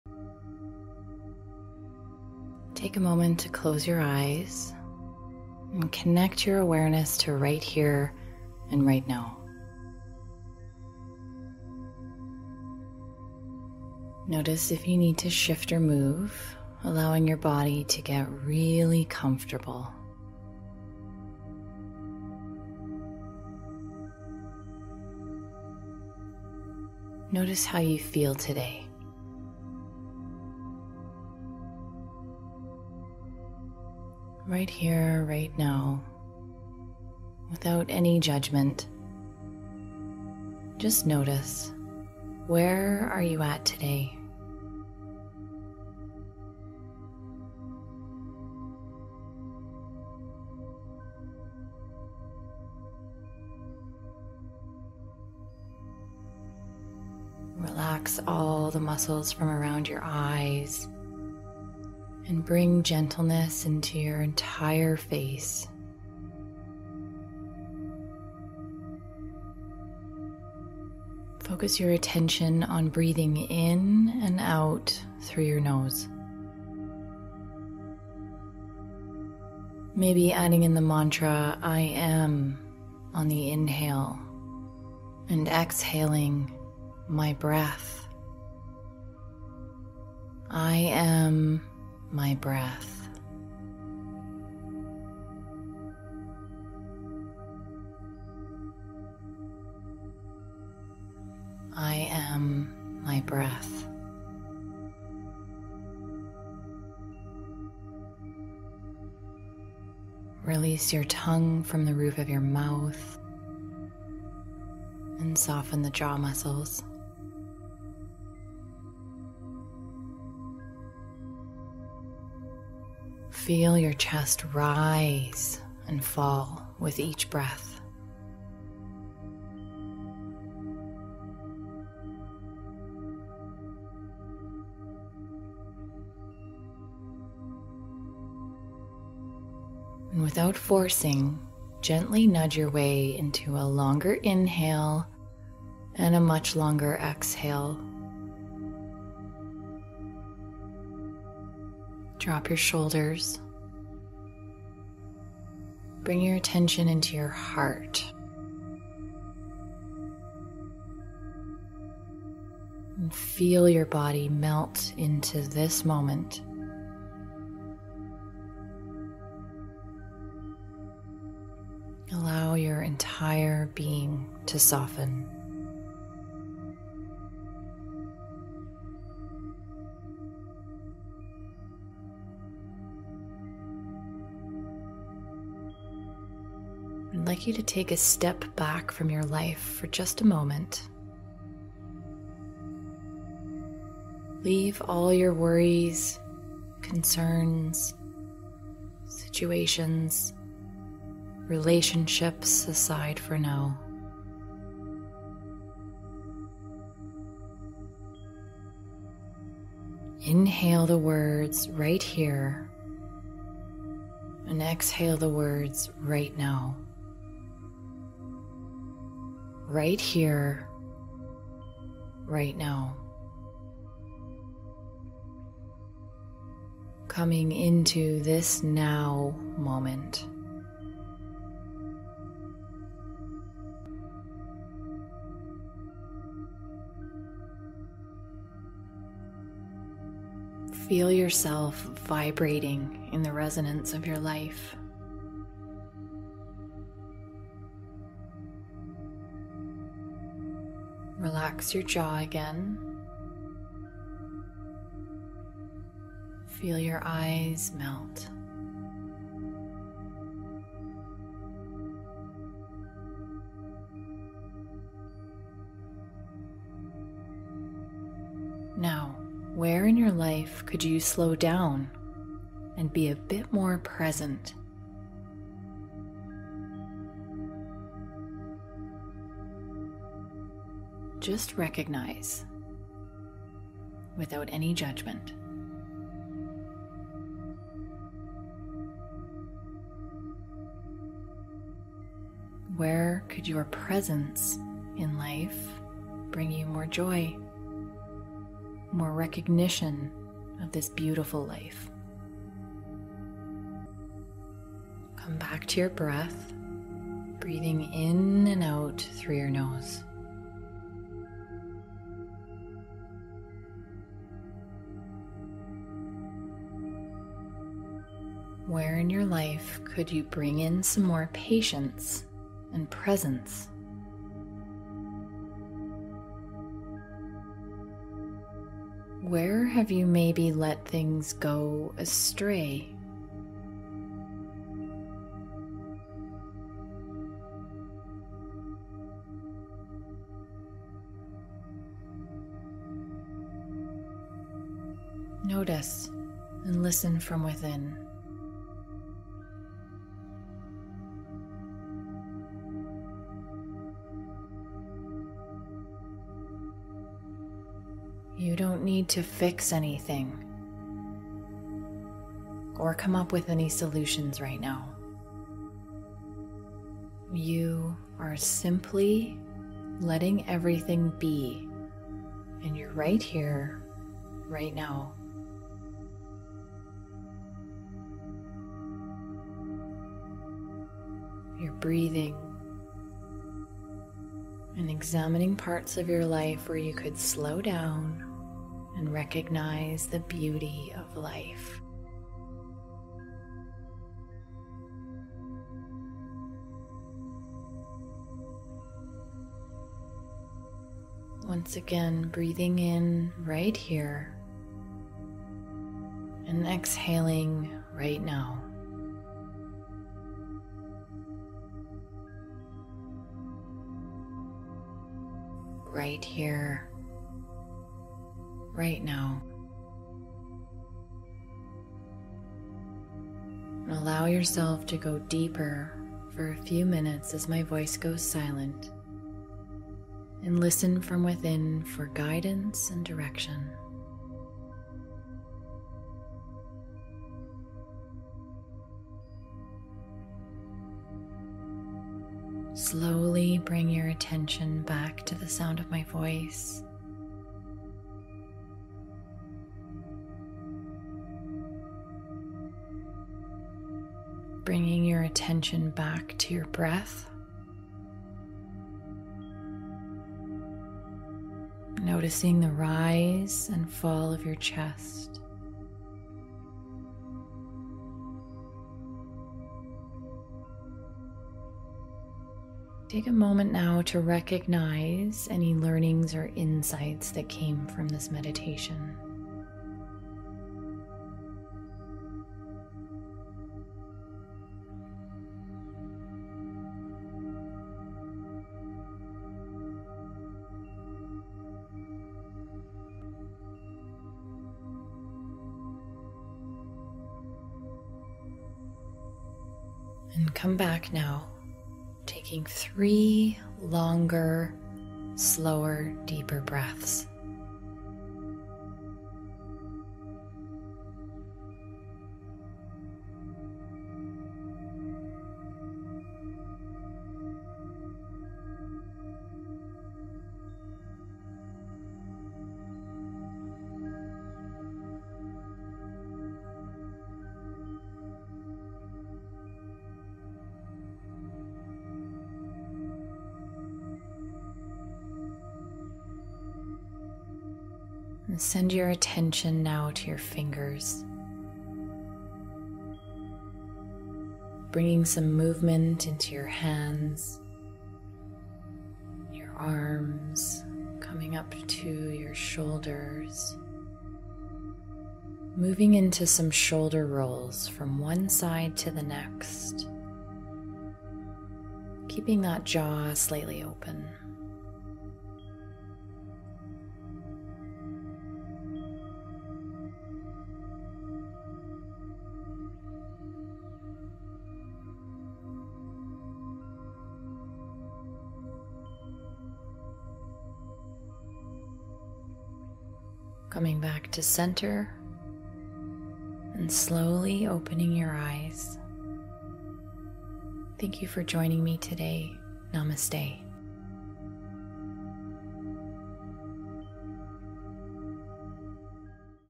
10 Minute Mindfulness Meditation | For Patience | Meditarium
10 Minute Mindfulness Meditation | For Patience | Meditarium
Mindfulness is a type of meditation in which you focus on being intensely aware of what you're sensing and feeling in the moment, without interpretation or judgment. Practicing mindfulness involves breathing methods, guided imagery, and other practices to relax the body and mind and help reduce stress.